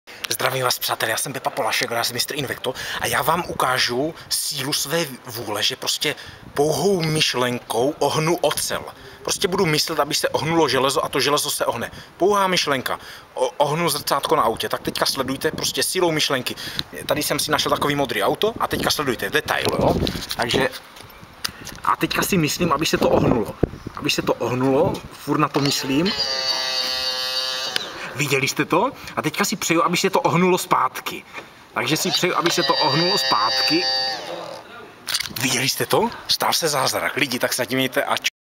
Síla myšlenky by mr. Invekto...
Ohnout pouhou myšlenkou železo pro mr. Invekto žádný problém... :)